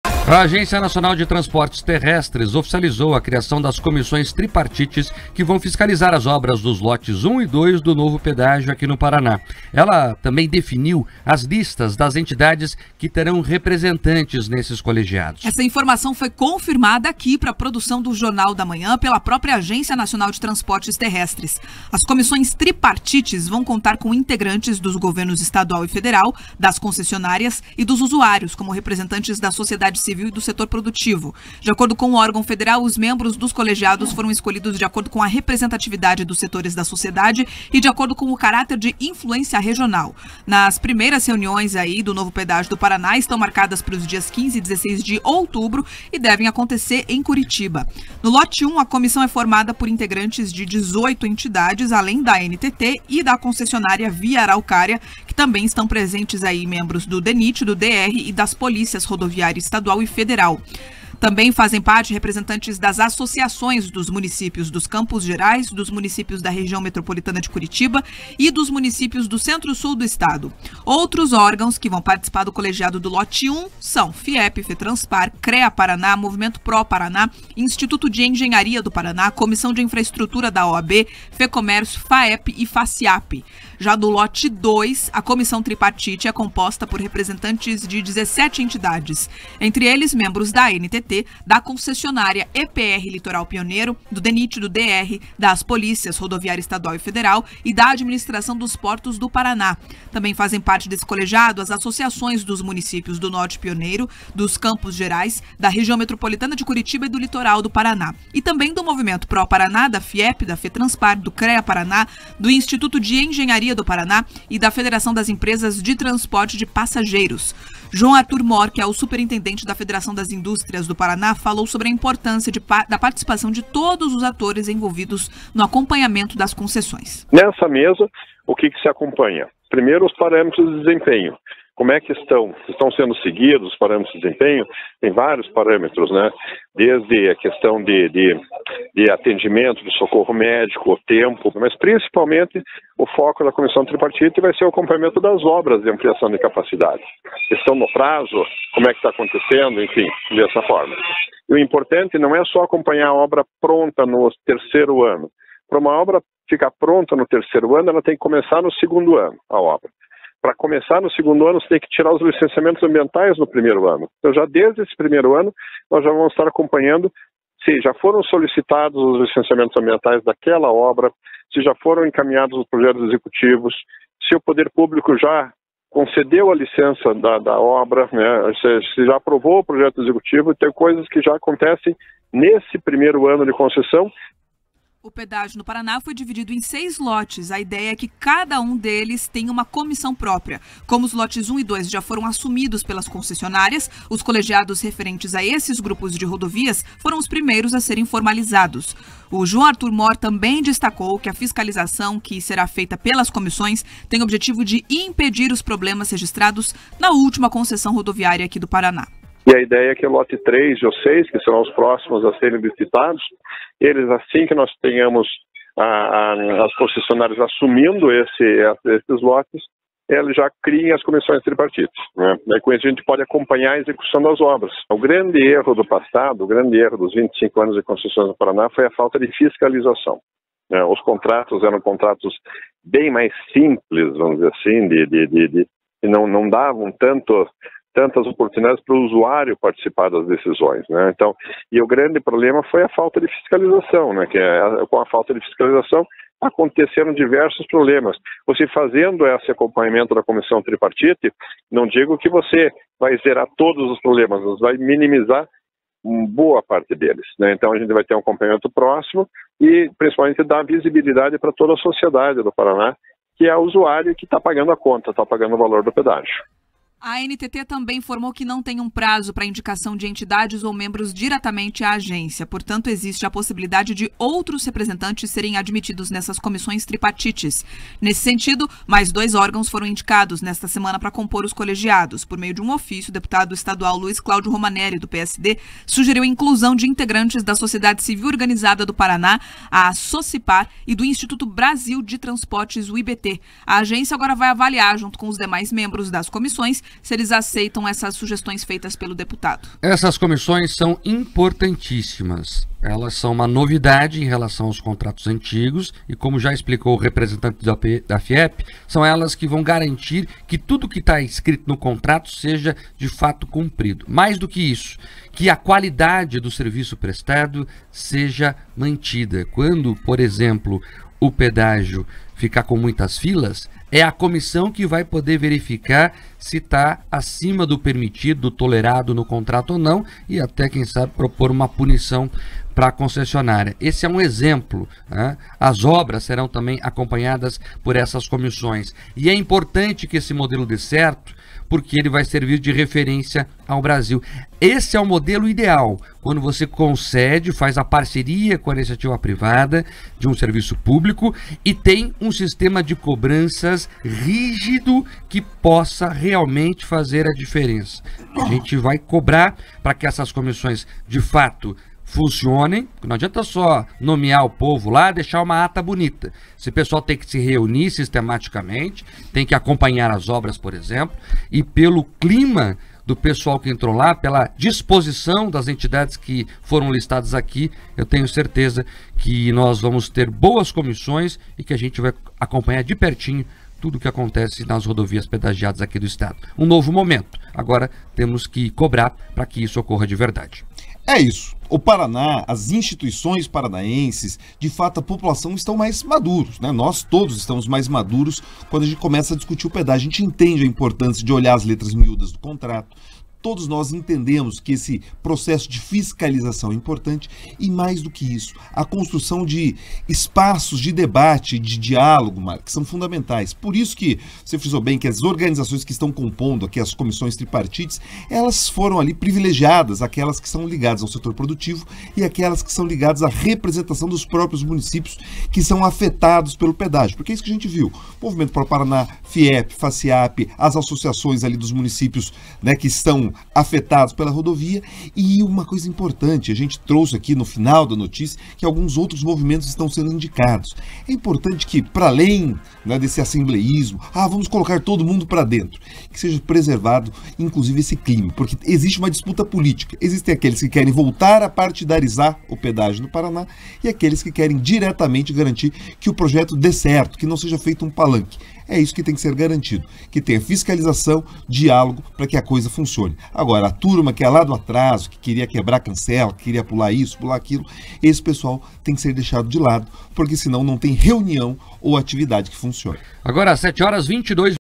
Pedágio: ANTT define membros de comissões tripartites
A Agência Nacional de Transportes Terrestres oficializou a criação das comissões tripartites que irão fiscalizar as obras dos lotes 1 e 2 do novo pedágio aqui no Paraná. Ela também definiu as listas das entidades que terão representantes nesses colegiados.